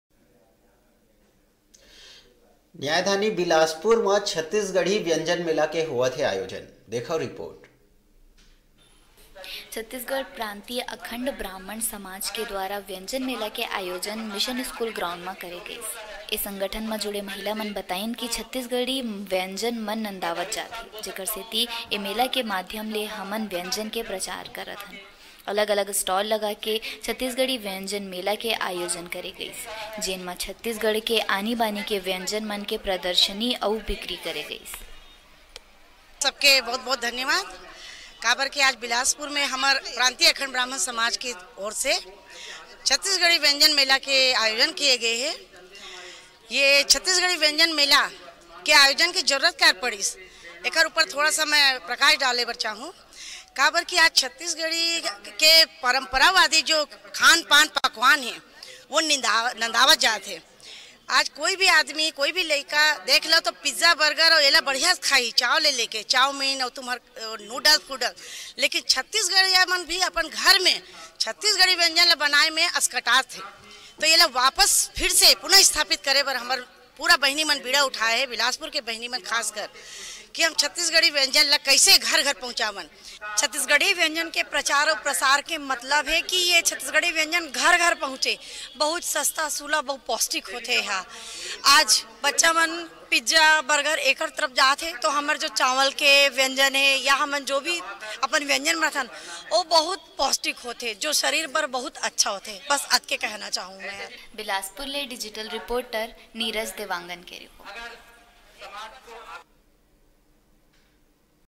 बिलासपुर म छत्तीसगढ़ व्यंजन मेला आयोजन
बिलासपुर ले नीरज देवांगन के रिपोर्ट